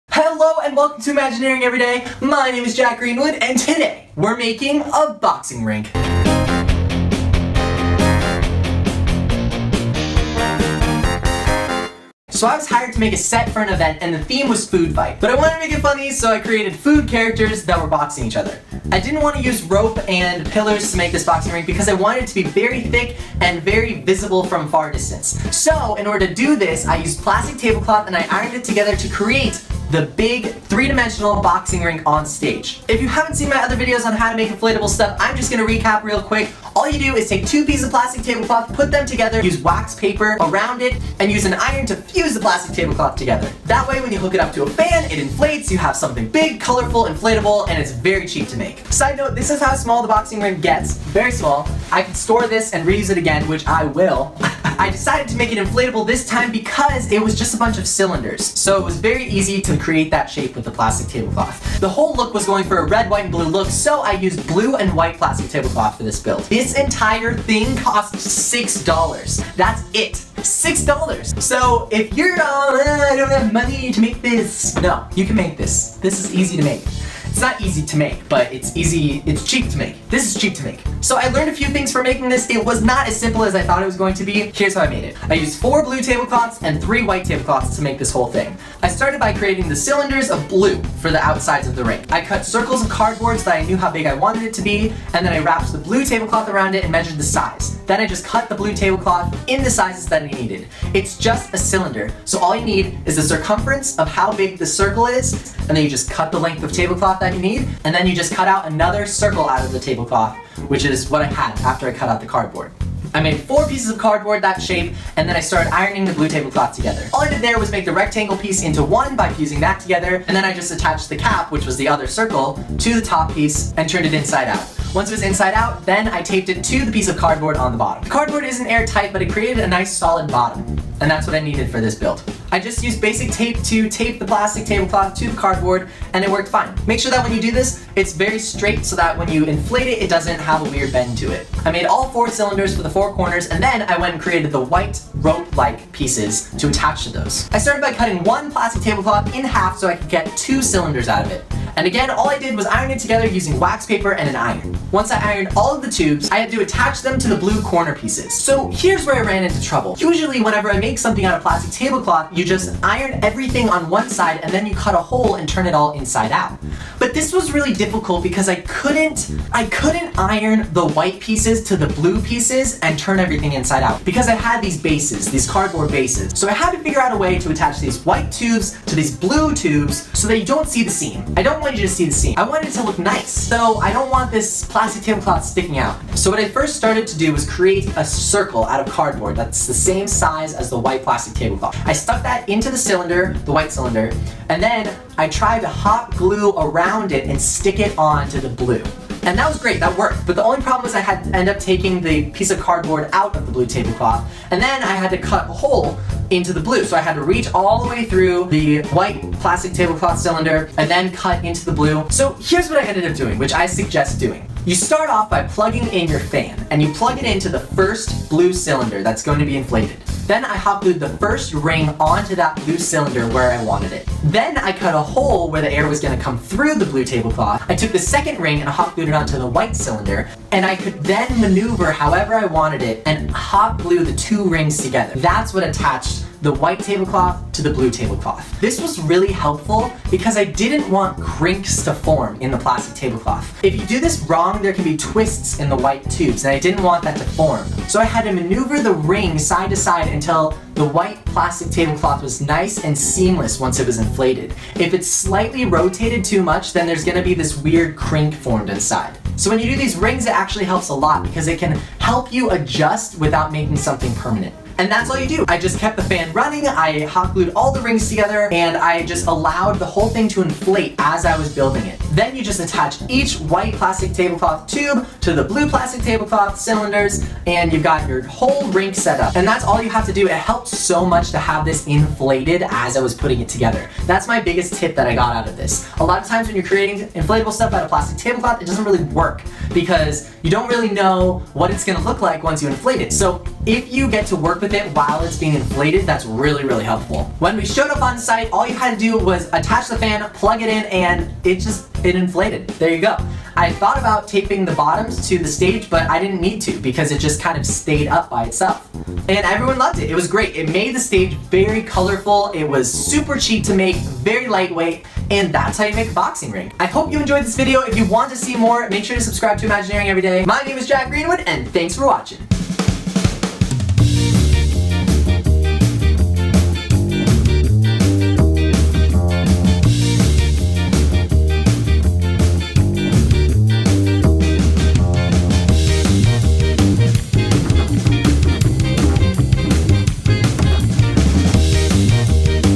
DIY Inflatable Boxing Rink
I got hired to make a set for an event themed food fight. The stage took a spin on a boxing tournament and i needed to create a large boxing rink. This is how I made it.

All I used to make this was:

 3 packages of blue plastic table cloth
and 3 packages of white

Both of which you can find at your local dollar store.